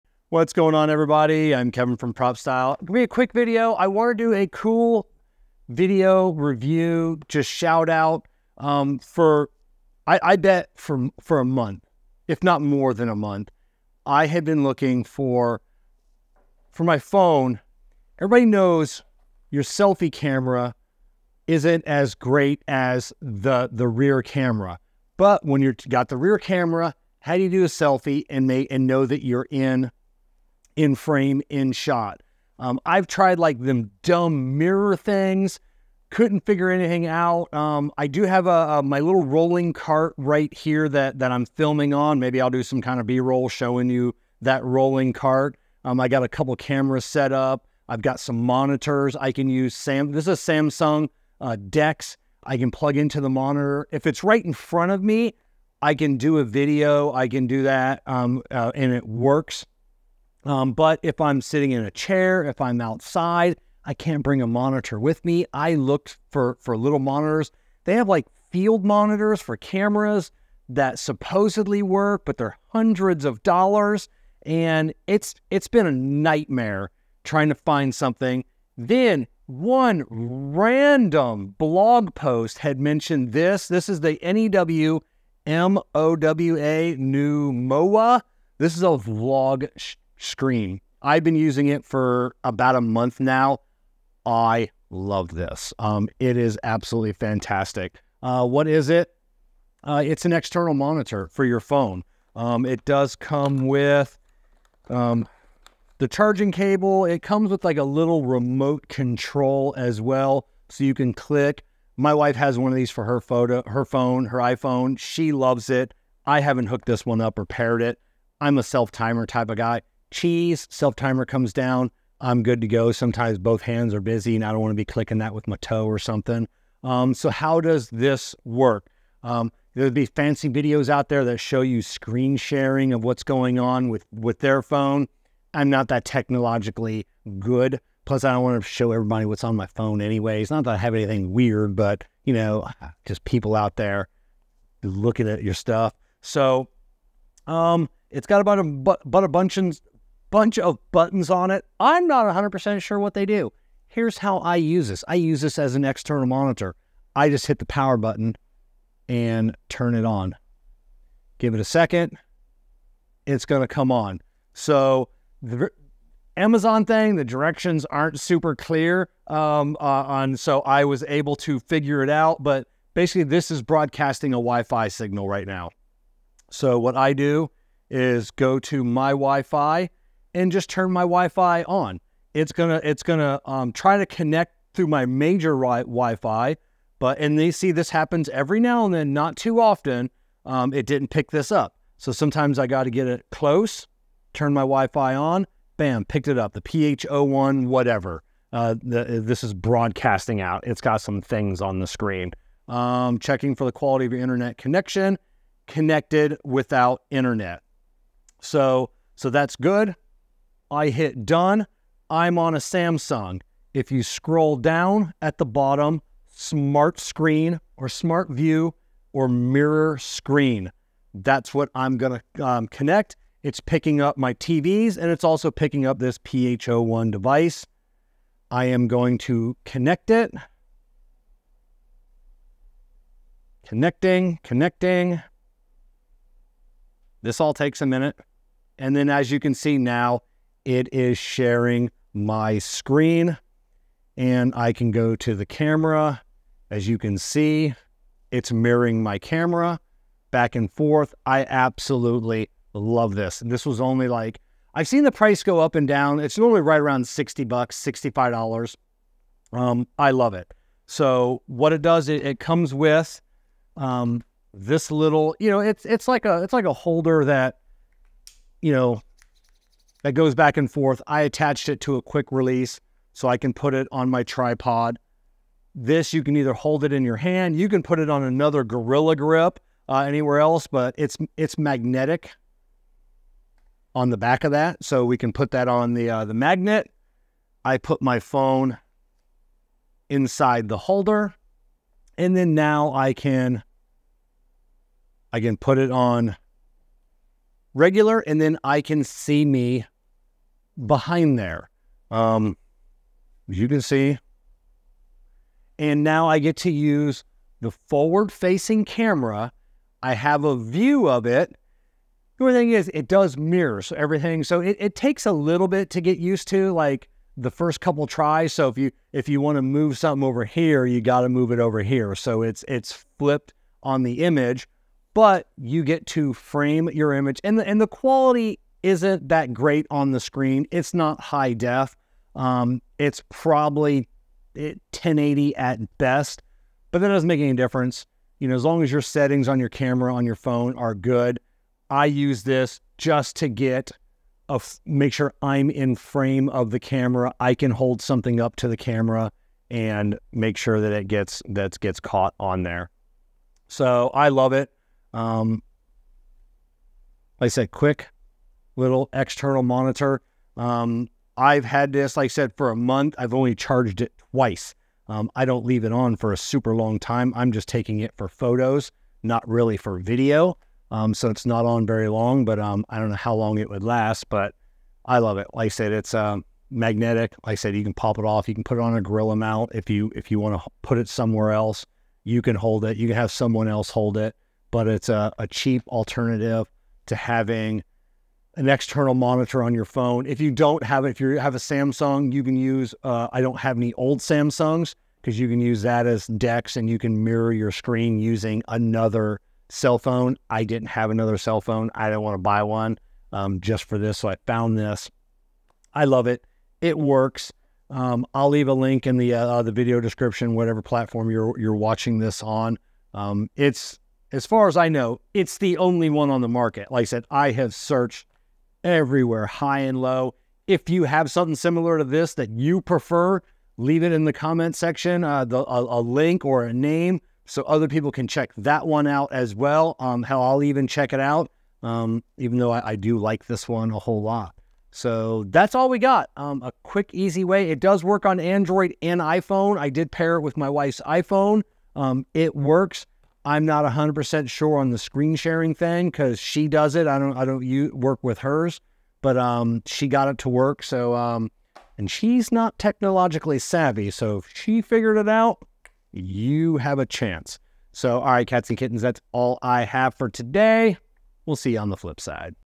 Vlog Selfie External Monitor Screen for Mobile Phone | PropStyle 2024
Welcome to the PropStyle YouTube Channel.

Unlock the full potential of your smartphone camera with the Newmowa External Monitor for Android and Apple devices! Tired of compromising on picture quality with the front-facing camera?
Say goodbye to subpar selfies and hello to crystal-clear shots and videos using your rear camera. With this external monitor, you'll always be perfectly framed for that ideal shot. Upgrade your photography and videography game today!

In this video, I talk about the Newmowa Phone Vlog Selfie Monitor Screen, Magnetic Phone Holder Clip Mount, Using Phone Rear Camera for Selfie Vlog Live Stream TikTok, Compatible with iPhone and Android that is sold on Amazon

Simpler Hair and Beard Color, I use it, and so should you!

Click here to sign up for our monthly newsletter.